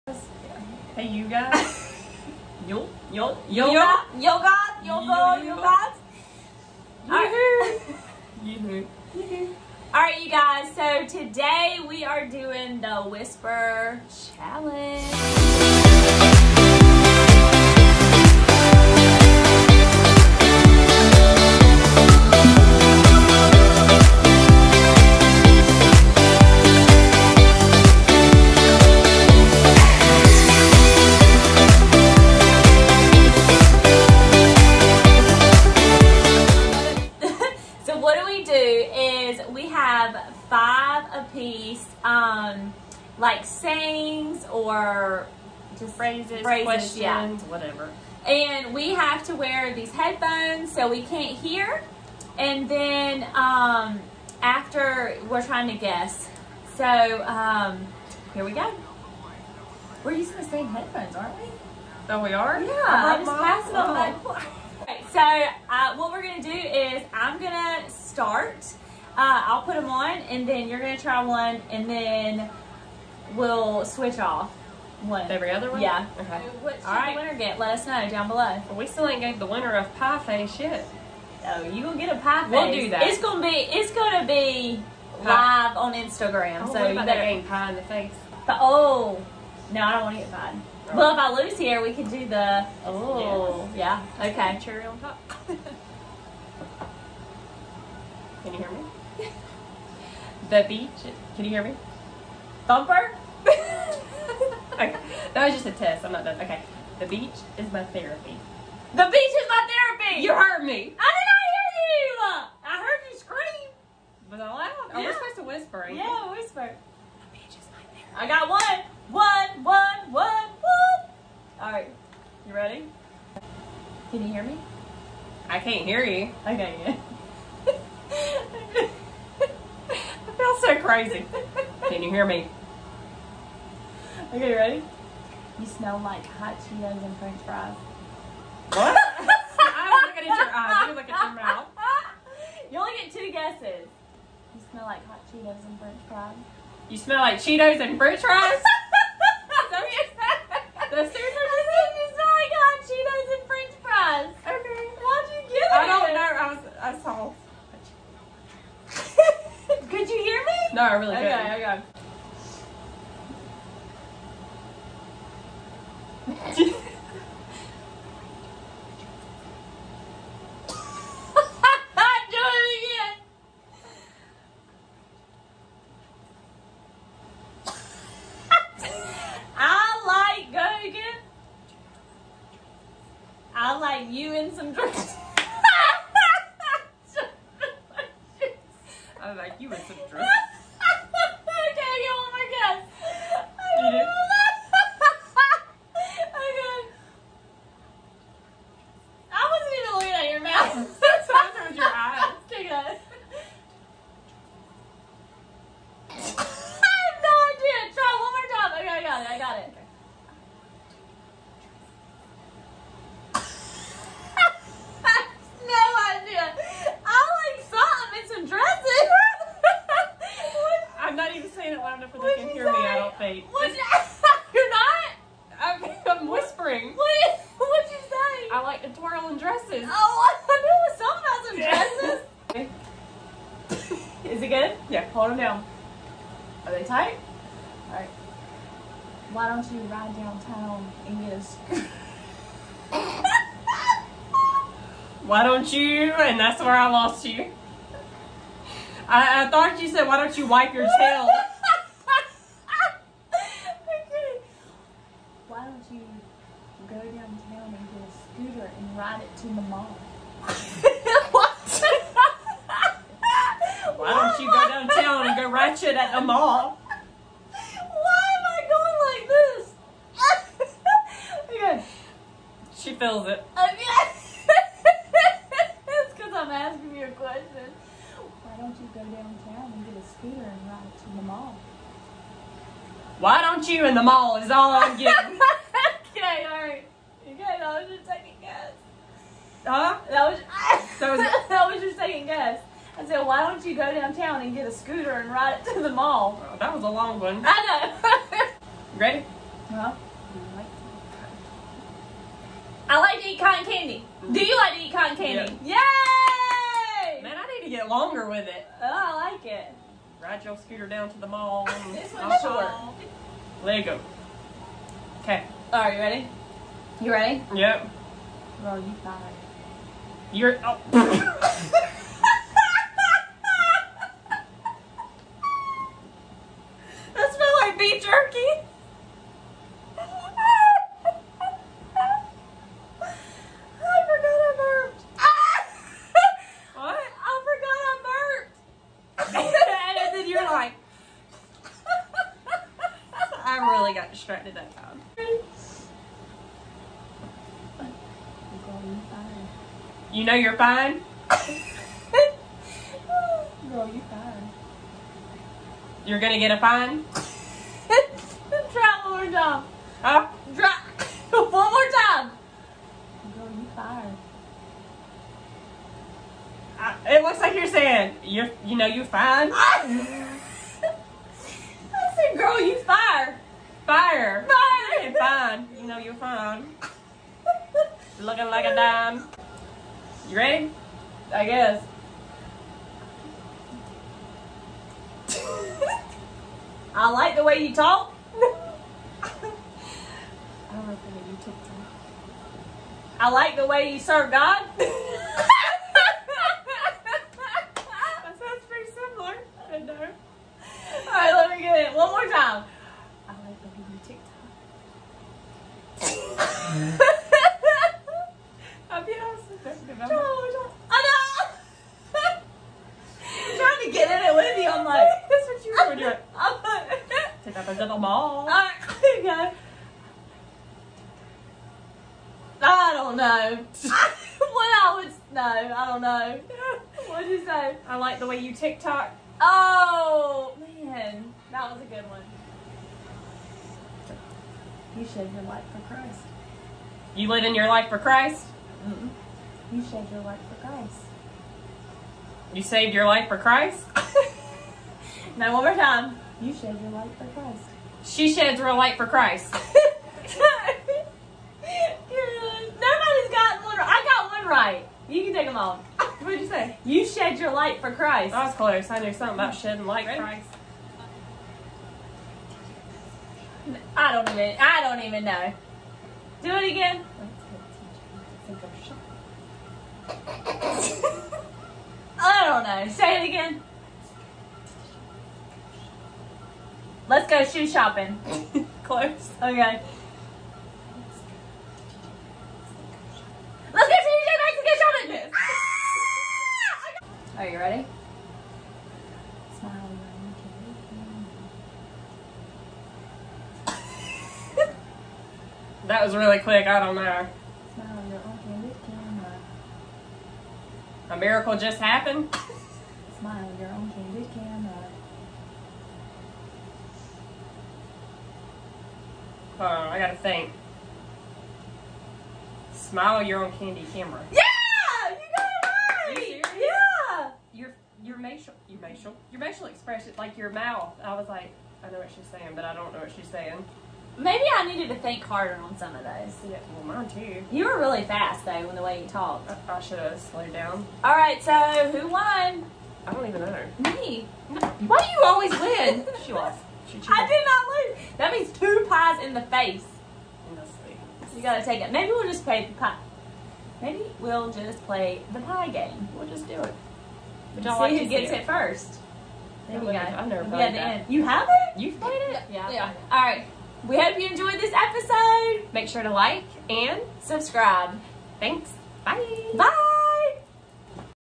The Whisper Challenge **Hilarious**
We try to figure out what each other is saying while listening to some of our favorite music!  Of course not without a ton of laughter and a lot of misunderstood phrases.

Instagram.com/sheshedsreallight